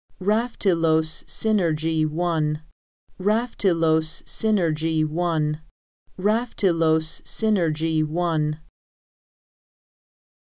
Pronounce Medical Words ― Raftilose Synergy 1
This video shows you how to say Raftilose Synergy 1.

How would you pronounce Raftilose Synergy 1?